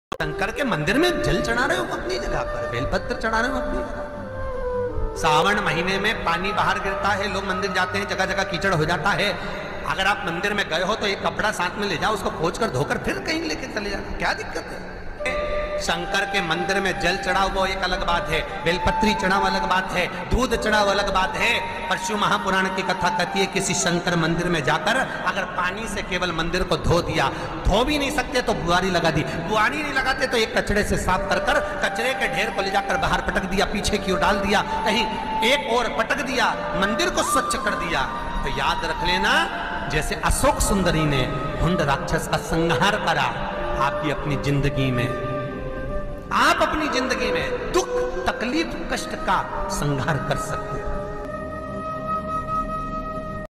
सावन का महीना शुरू होते ही प्रत्येक सोमवार के दिन शिव मंदिर में ये एक काम जरूर करें| shivpuran upay
pandit pradeep mishra ki aaj ki shiv mahapuran katha
pandit pradeep mishra ki aaj ki shiv puran